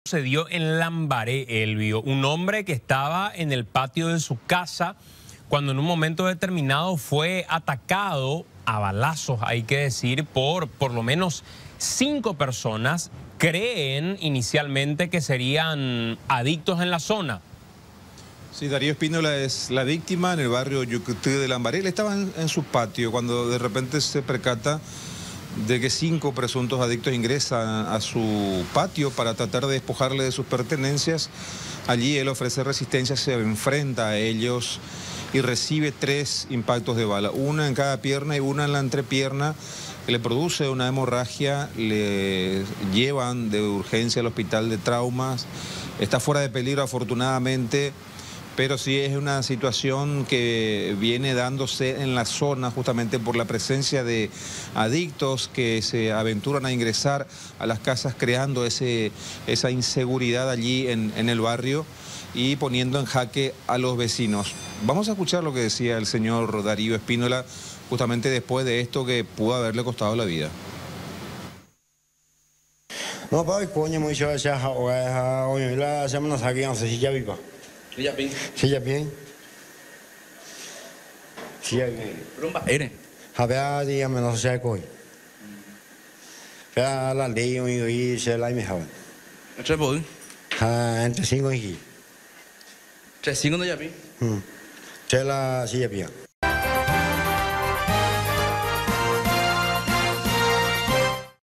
Baleado por asaltantes en su propia casa
Un hombre que estaba en el patio de su vivienda ubicada en Lambaré fue atacado por cinco presuntos adictos que intentaron despojarlo de todas sus pertenencias. La víctima se resistió y los sujetos le balearon en la pierna.